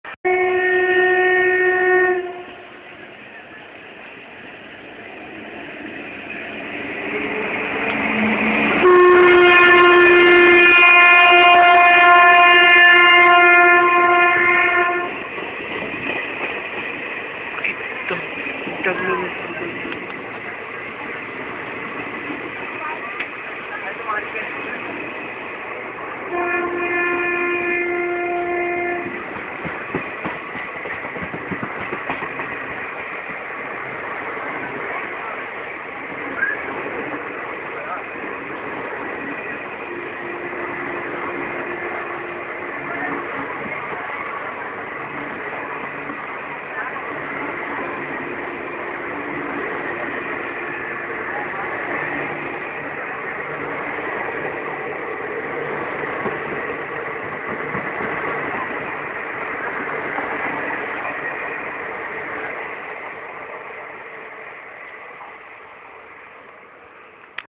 HWH WAP 4 22862 ENJOYING MONSOON WITH W.B. SAMPARK KRANTI............
28/05/2013: HWH WAP 4 22862 CRWALS THROUGH DANKUNI JN AMIDST HEAVY RAIN WITH 12329 UP SEALDAH - DELHI WEST BENGAL SAMPARK KRANTI EXP AT ITS TOW.